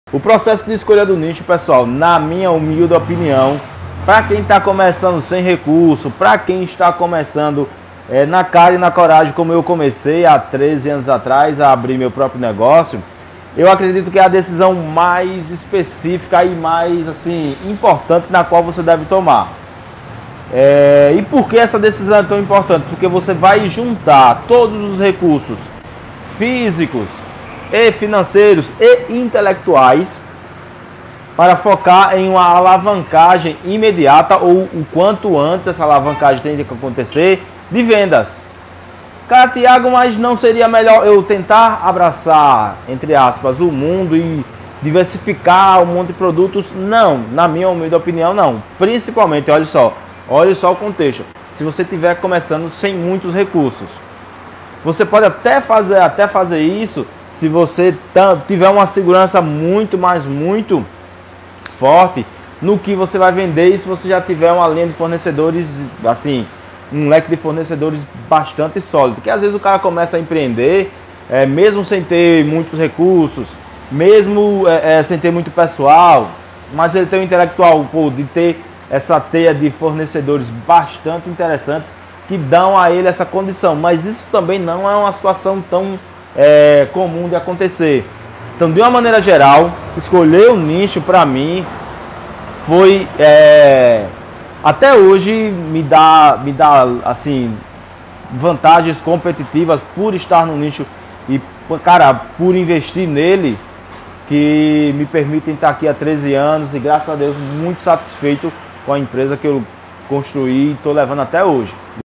Ampliando a visão de como começar sua gráfica rápida!
Nesse vídeo exponho minha visão sobre a escolha de um nicho de atuação dentro do universo de uma gráfica rápida, para se obter melhores resultados e se tornar mais competitivo.